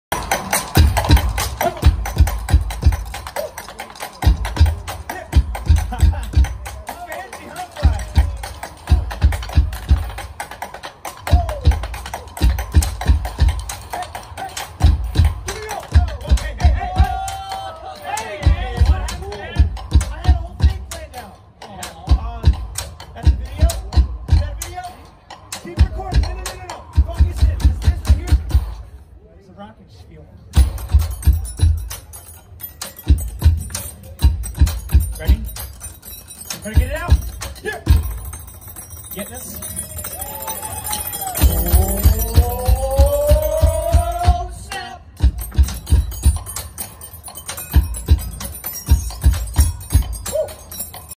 Full Beat Builders street show at Universal Studios Florida, January , 2019
At Beat Builders, four construction workers working on a new project use their tools and other objects around to create some catchy beats.